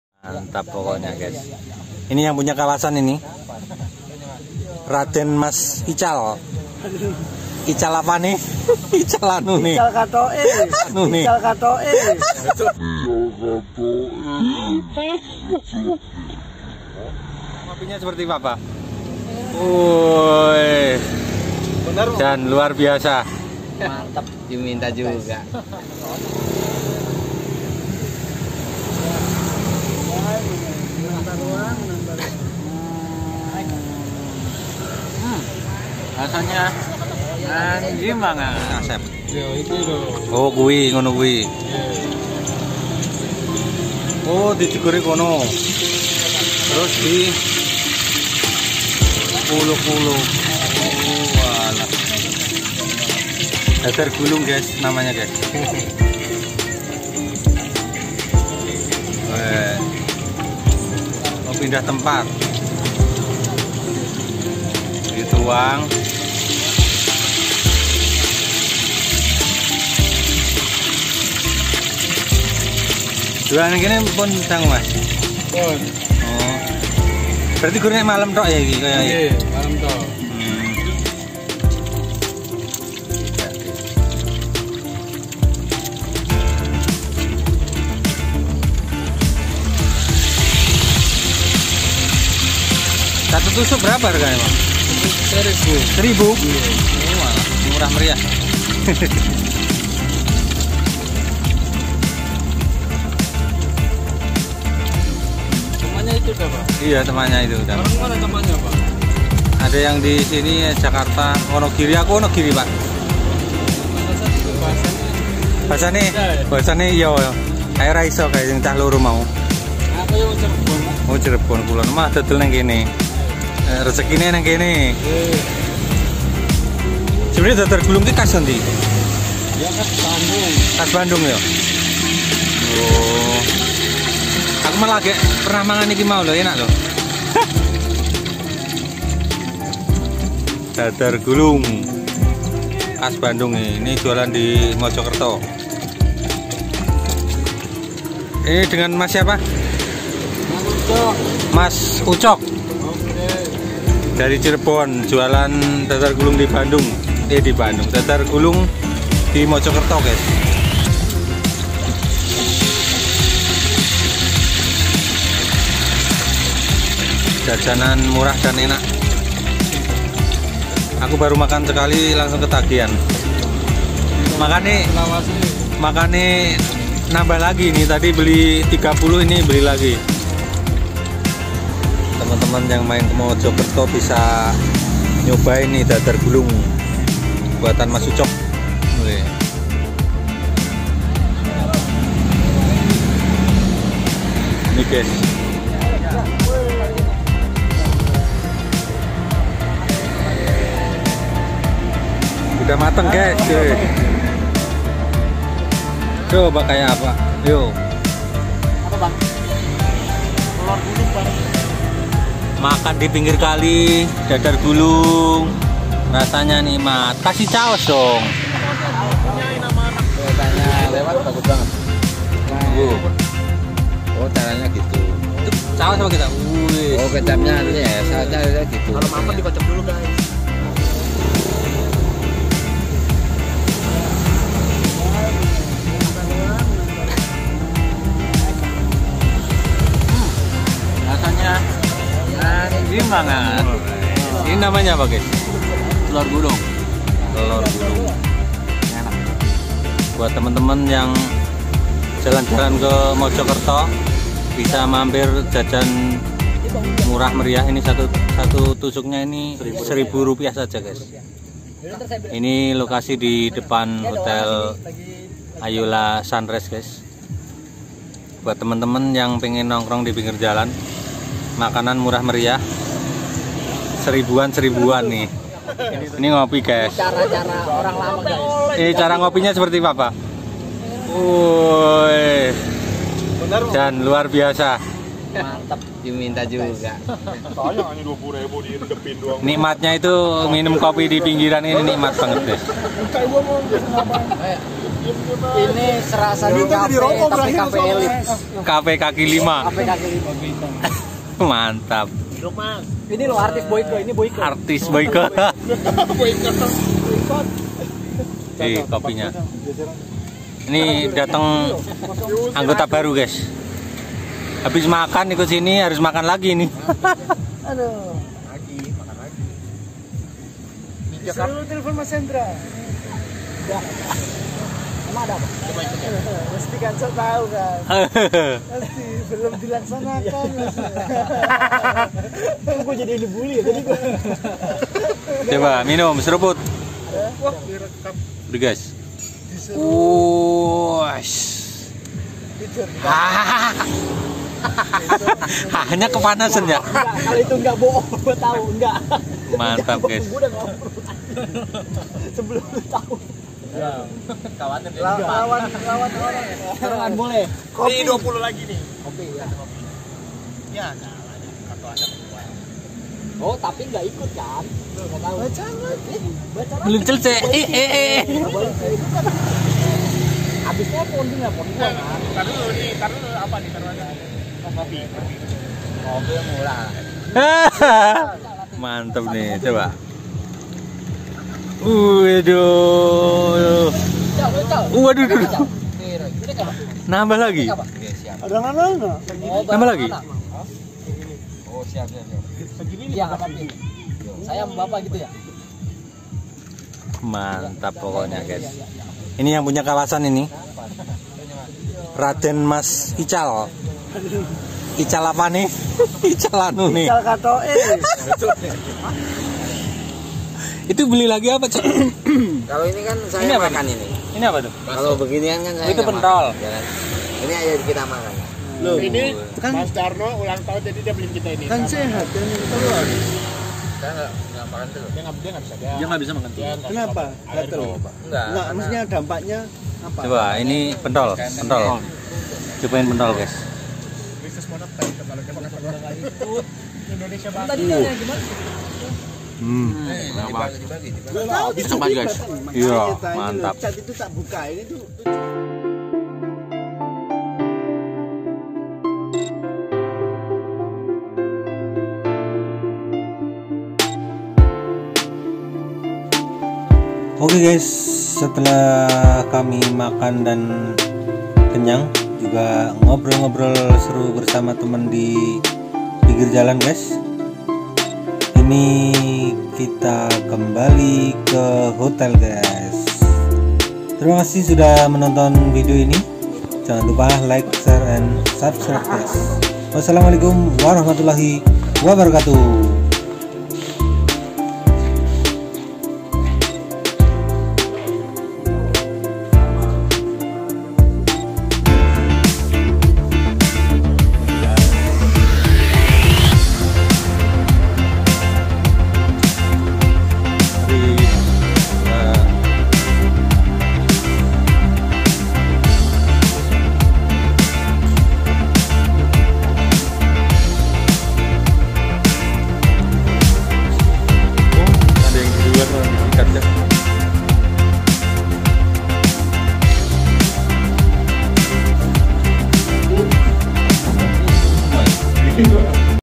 cara mudah membuat telor gulung jajanan murah cuma 1.000 an
Halo temanku semua, di video kali ini Cah Bejo Wonogiri bersama teman-teman lagi nongkrong bareng sambil menikmati jajanan murah meriah cuma seribuan yaitu telor gulung, meski murah namun nikmat lho, jika kalian main ke Mojokerto Jawa Timur bisa nyobain kalau malam hari tempatnya ramai sekali lokasi persis di depan hotel ayola sunrise Mojokerto, di sepanjang jalan banyak yang berjualan beraneka macam jajanan kalian tinggal pilih di jamin bahagia gaess hehe,